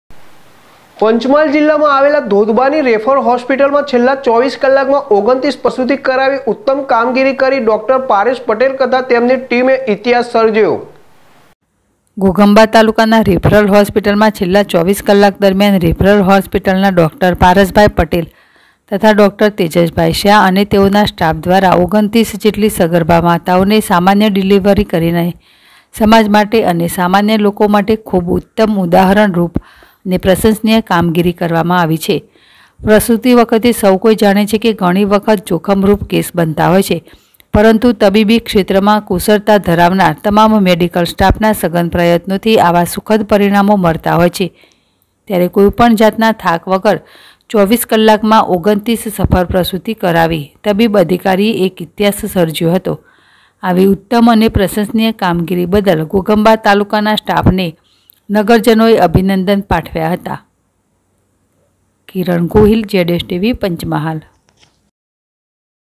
પંચમહાલ જીલામાં આવેલ ઘોઘંબાની રેફરલ હોસ્પિટલમાં છેલ્લા 24 કલાકમાં 29 પ્રસુતિ કરાવીને ઉત્તમ
પંચમહાલ જીલામાં આવેલ ઘોઘંબાની રેફરલ હોસ્પિટલમાં છેલ્લા 24 કલાકમાં 29 પ્રસુતિ કરાવીને ઉત્તમ કામગીરી કરી ડો. પારસભાઈ પટેલ તથા તેમની ટીમે ઇતિહાસ સર્જ્યો.કહી શકાય